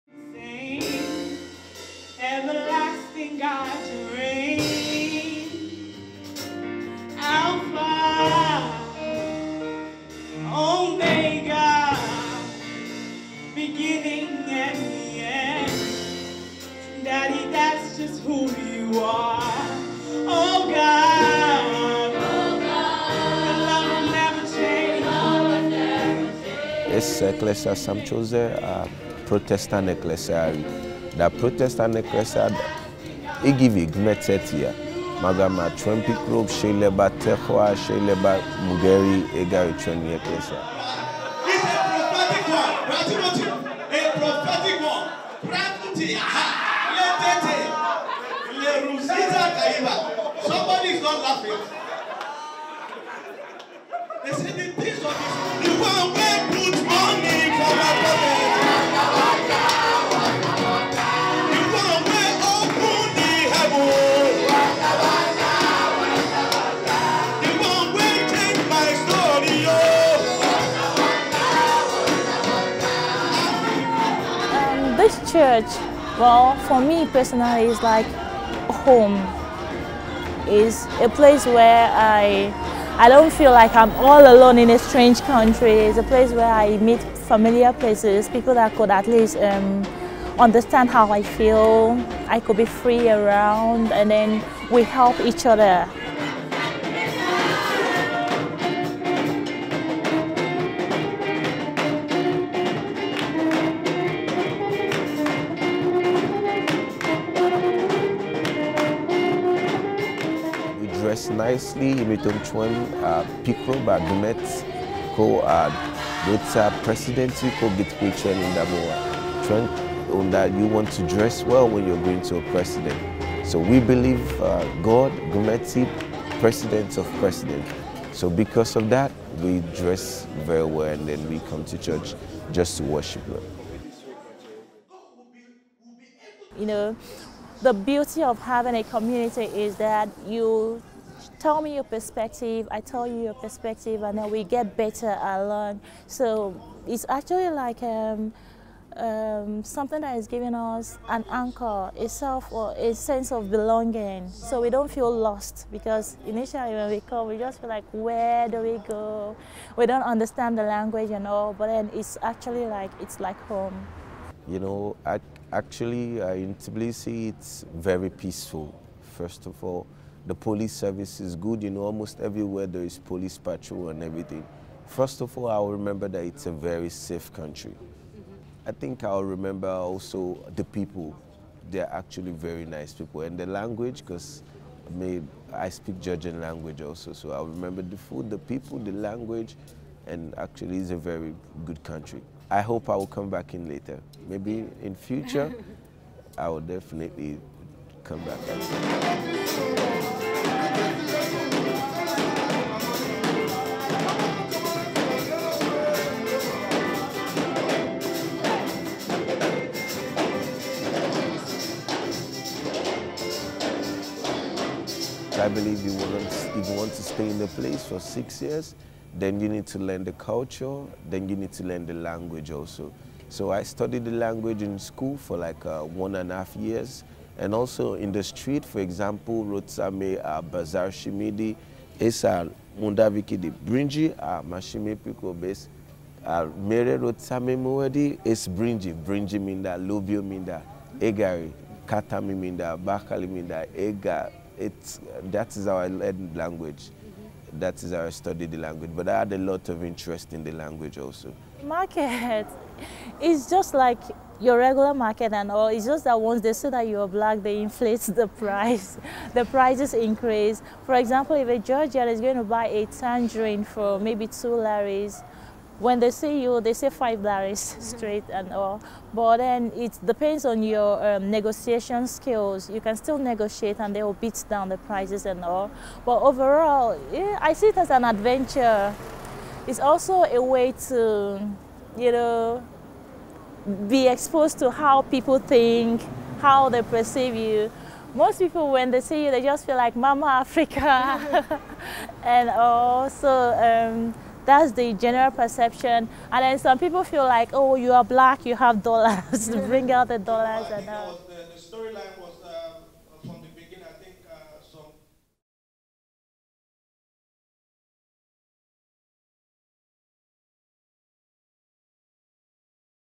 ნიგერიიდან თბილისში
თბილისში, საბჭოს მოედანთან,  ერთ-ერთი საცხოვრებელი სახლის სარდაფში თითქმის 10 წელია ფუნქციონირებს დახსნილ ქრისტიანთა საღვთო ეკლესია, რომლის მრევლიც თავს პროტესტანად თვლის.